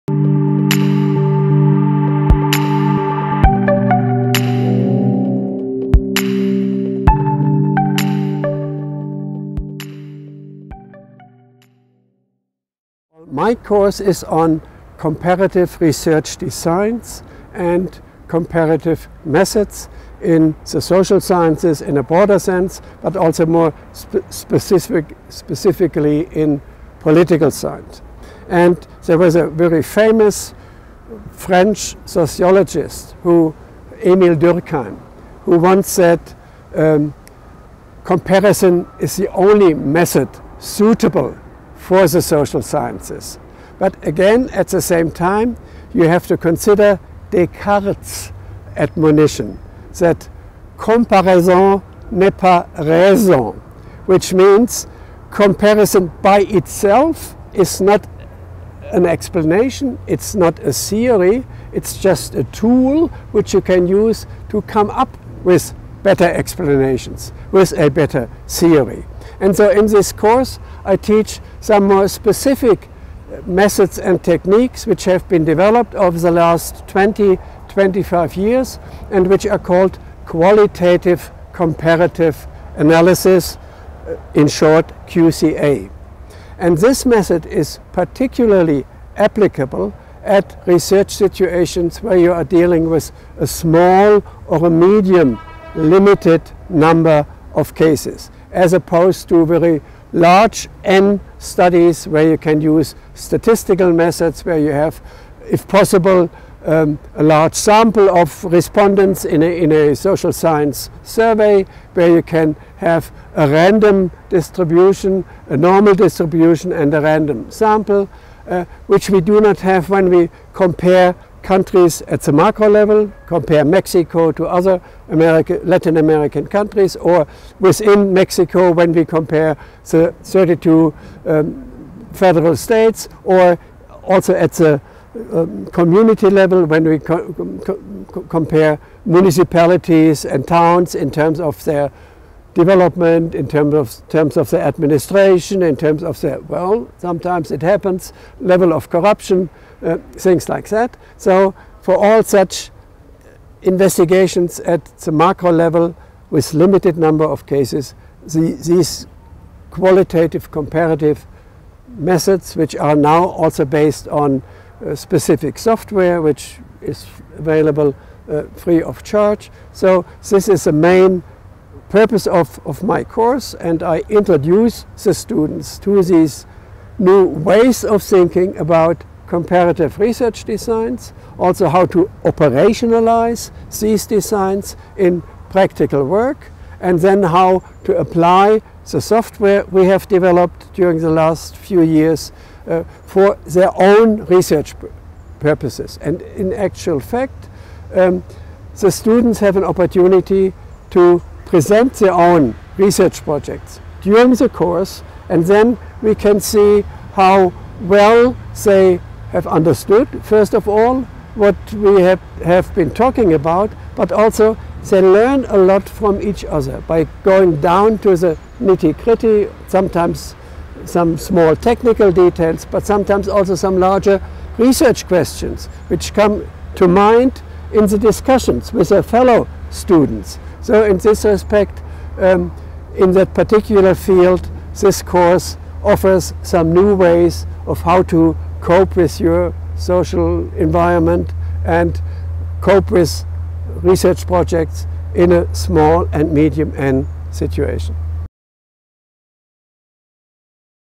Diseño de investigación comparada y métodos comparados - Dirk Berg-Schlosser
El curso Diseño de investigación comparada y métodos comparados a cargo del doctor Dirk Berg-Schlosser de a Universidad de Marburg en Alemania introduce al alumno a una visión conjunta de los distintos tipos de análisis comparados sistemáticos en las ciencias sociales, y se muestra cómo implementar este método para construir teorías y explicar de manera constructiva.